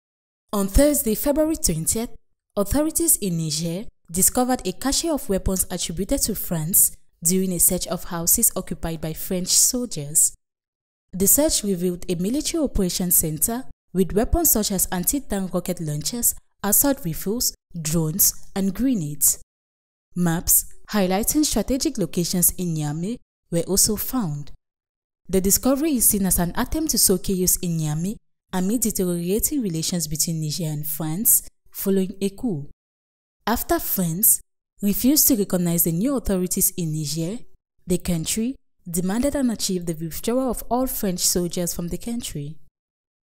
NIGER: AUTHORITIES FIND CACHE OF WEAPONS LEFT BY FRENCH TROOPS IN NIAMEY
On thursday February 20th, authorities in Niger discovered a cache of weapons attributed to France during a search of houses occupied by French soldiers.